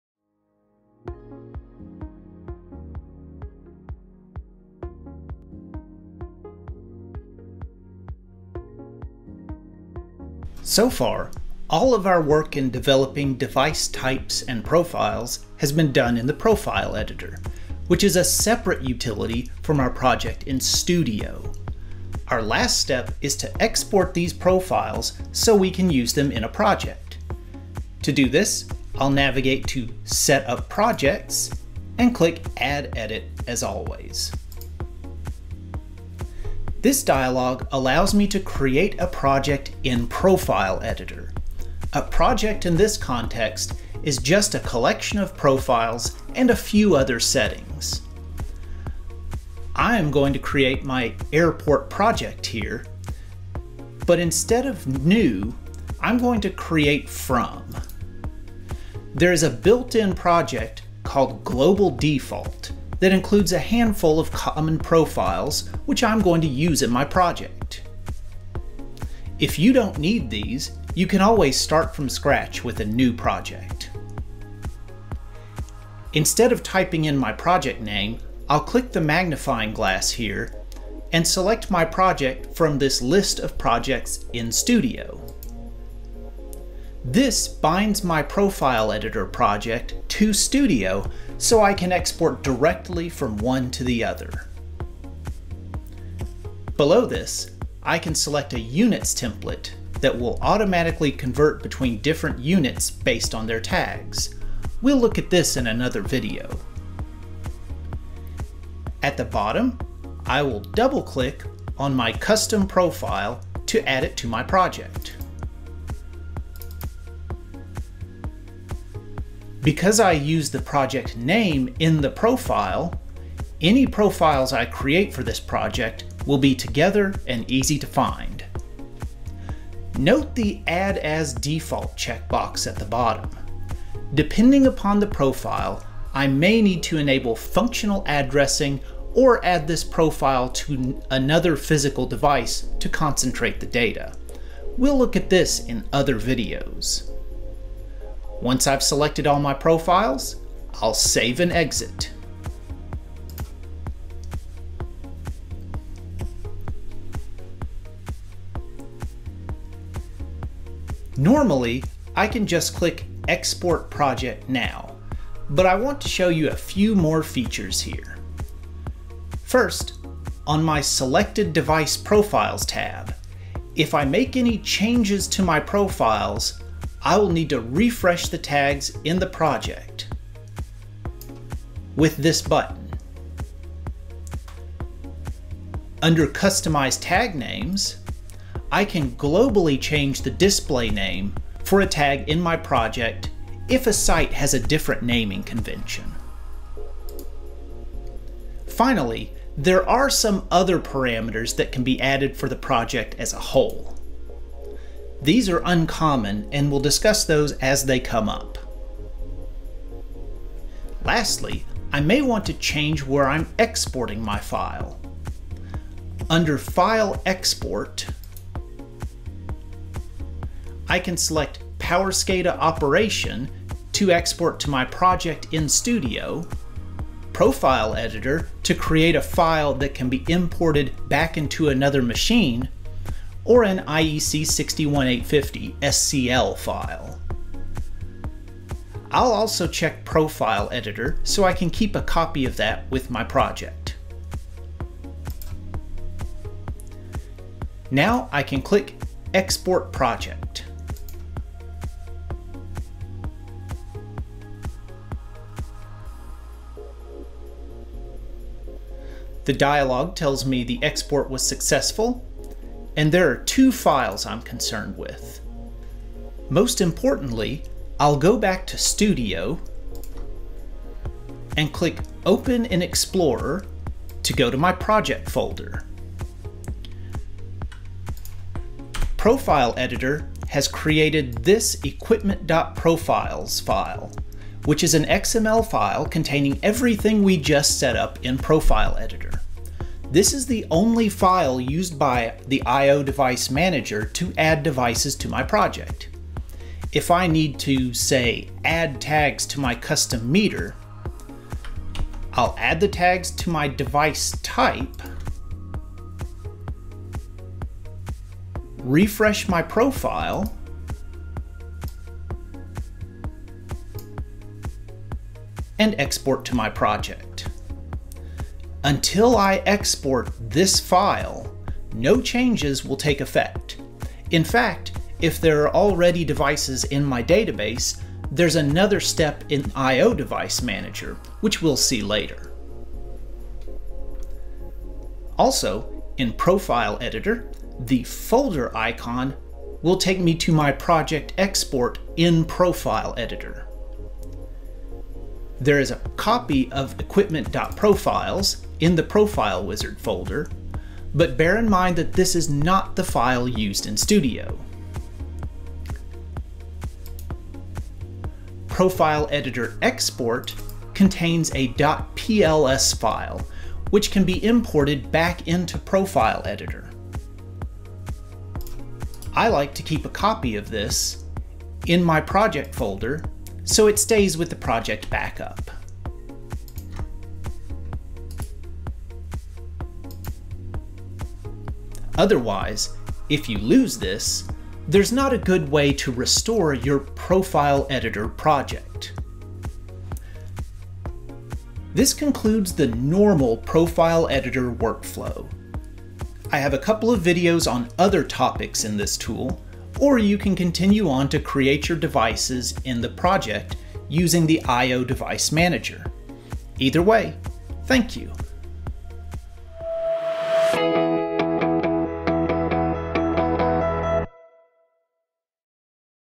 EcoStruxure Power Operation: Ch4 - Export Profiles to Your Project | Schneider Electric Support
As the last step in the engineering workflow of the Profile editor this video describes exporting the profiles so that you can start using them in your actual project. It also discusses the import and export options that can help share device profiles between multiple power operation systems. Post the export it discusses the folder contents that can help distinguish the option to export profiles within a project and outside a project.